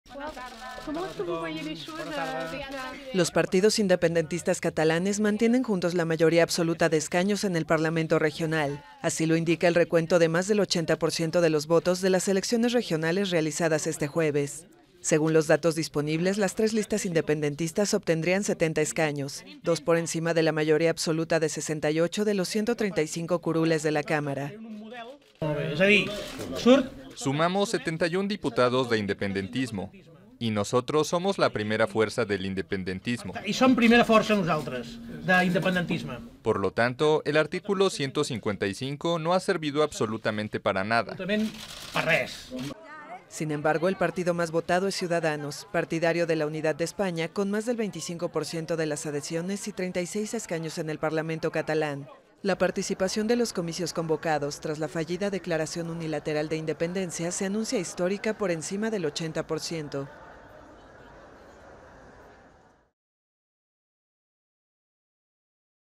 Independentistas catalanes obtienen mayoría absoluta de escaños
Los partidos independentistas catalanes mantienen juntos una estrecha mayoría absoluta de escaños en el Parlamento regional, aunque la lista más votada es Ciudadanos, partidaria de la unidad de España, tras el recuento del 84,3% de los votos.